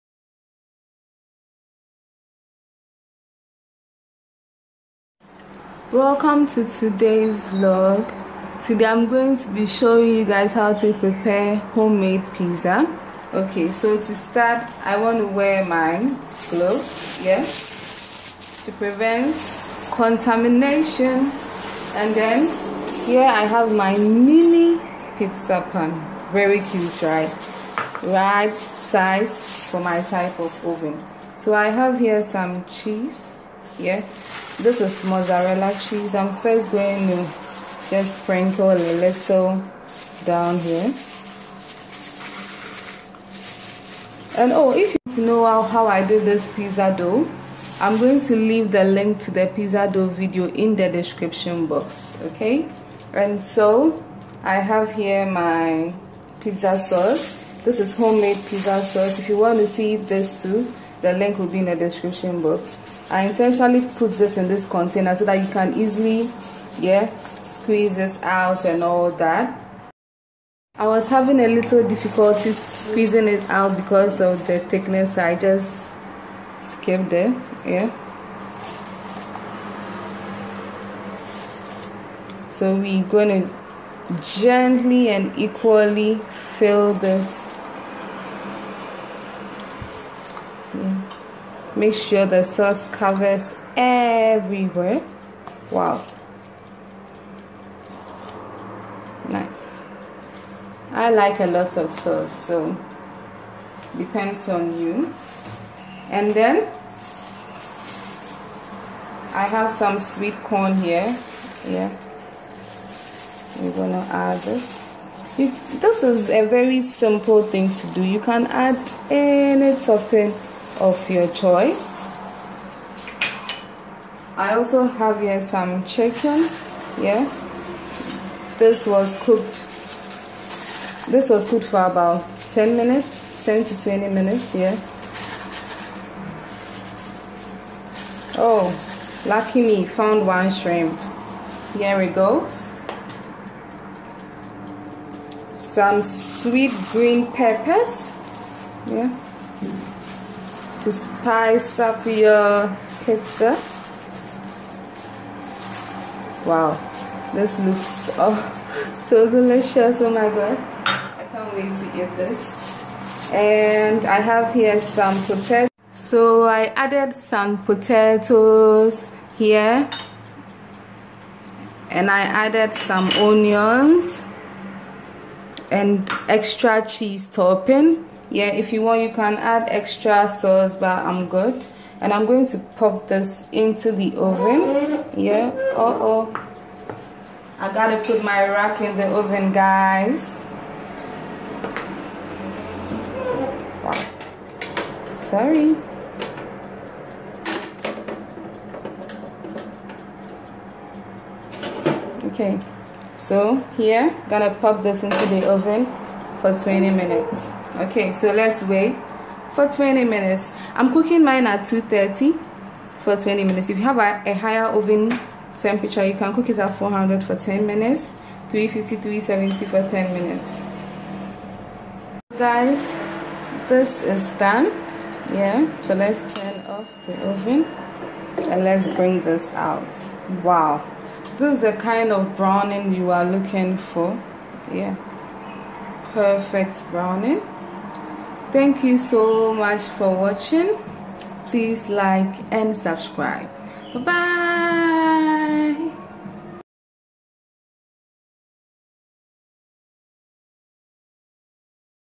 How to make homemade Pizza
INGREDIENTS
pizza dough
Pizza sauce
Green pepper
Cheese
Onions
Chicken
Potatoes